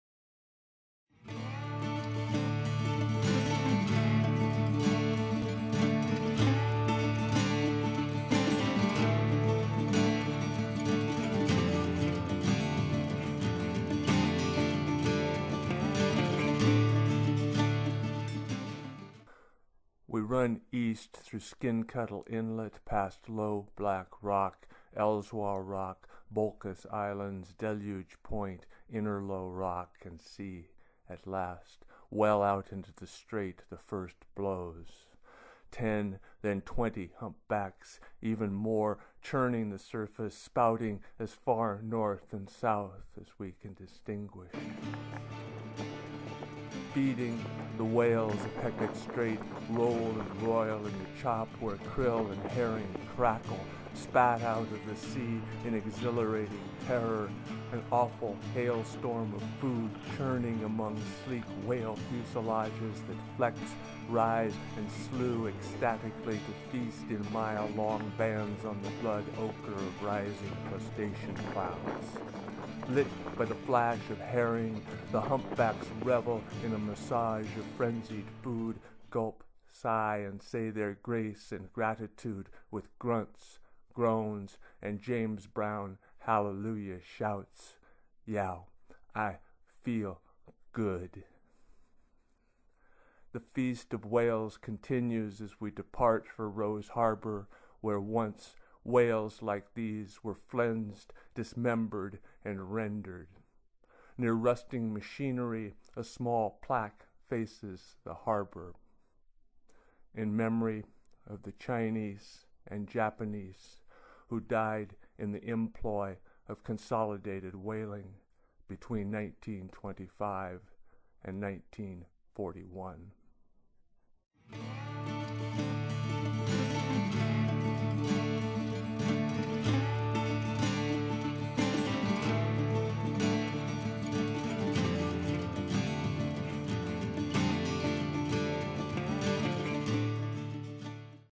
Feeding, the whales of Hecate Strait 2
Journal entries, short poem, photos and video in honor of the great whales (Humpbacks) of Hecate Strait off Haida Gwaii, formerly the Queen Charlotte Islands. Video from off the SE coast of Haida Gwaii. July 18, 2011. Scores of humpback whales feeding on krill and herring (?). An amazing and powerful sight. Thanks to Barrett Johnson for the video.